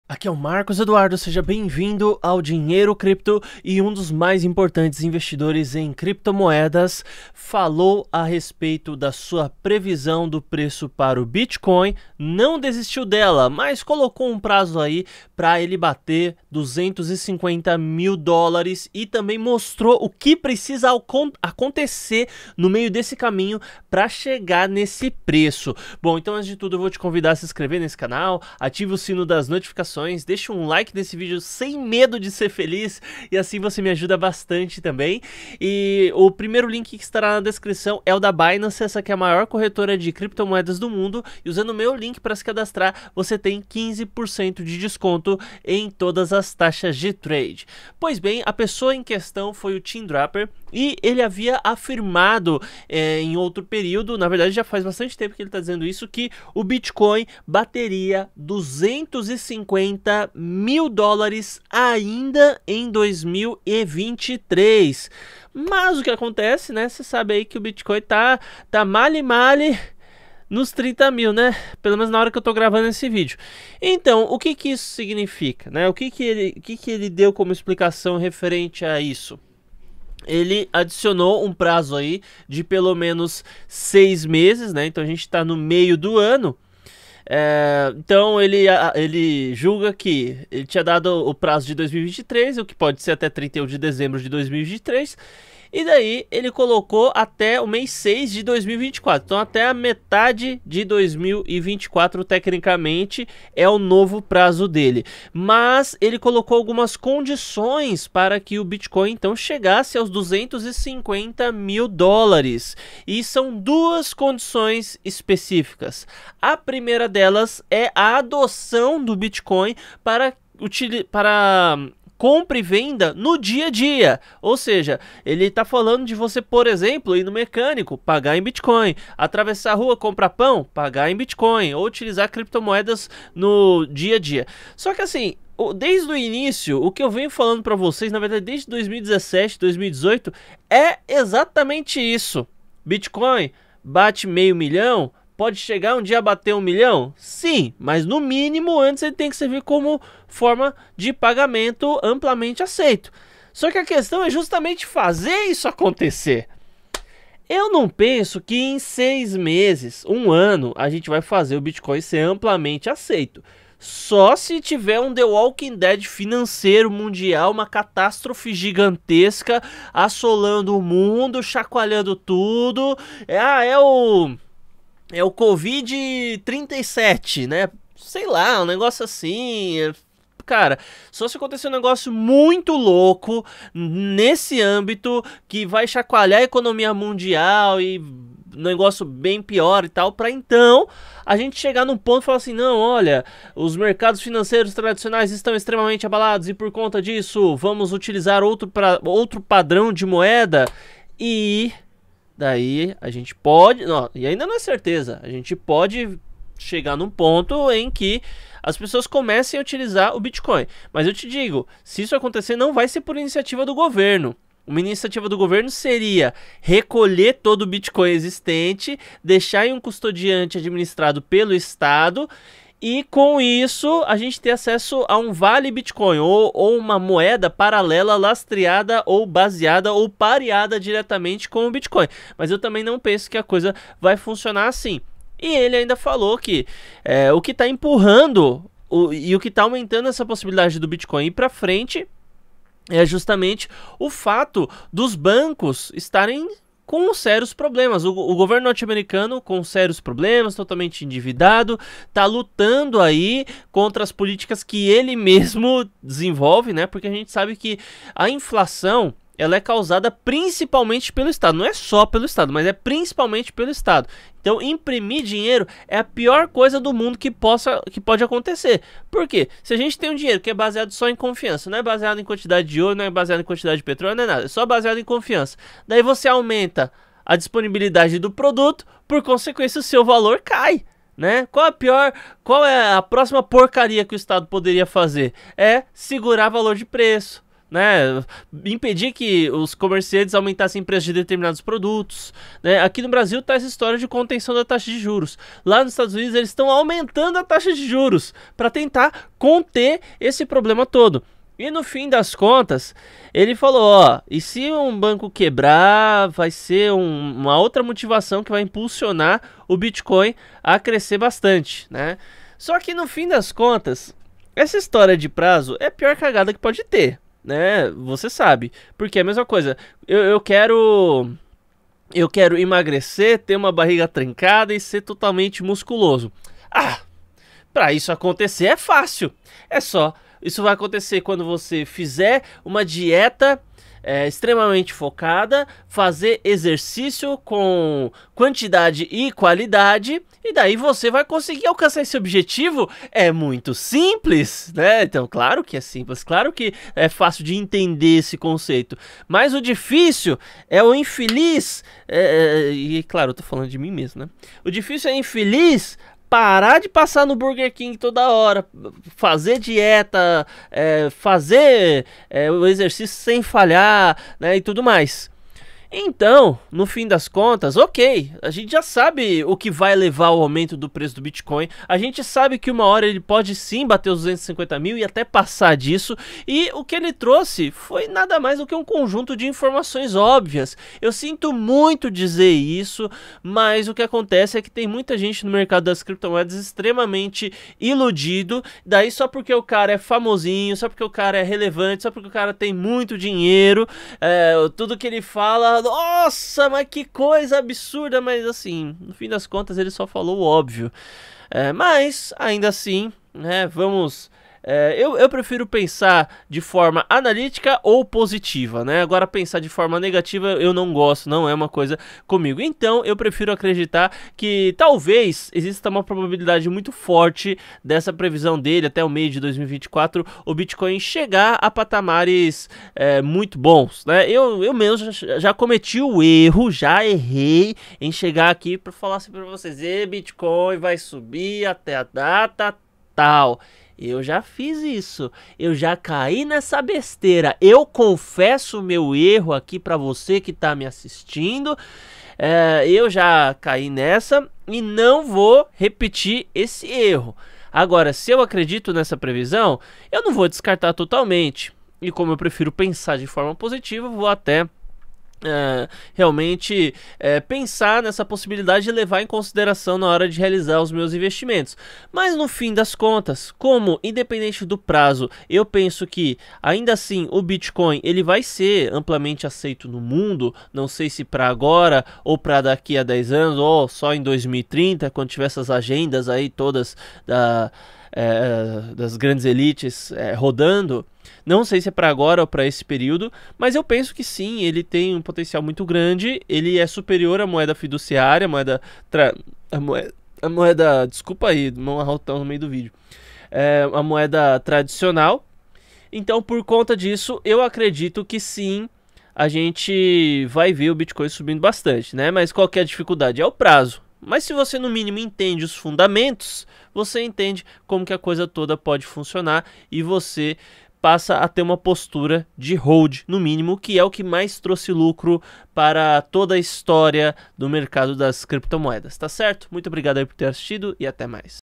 BITCOIN A CAMINHO DOS US$ 250 MIL? Previsão impactante de investidor no Brasil
Descubra a previsão ousada de um investidor no Brasil sobre o futuro do Bitcoin, com possibilidade de atingir US$ 250 mil! Acompanhe nosso vídeo e saiba mais!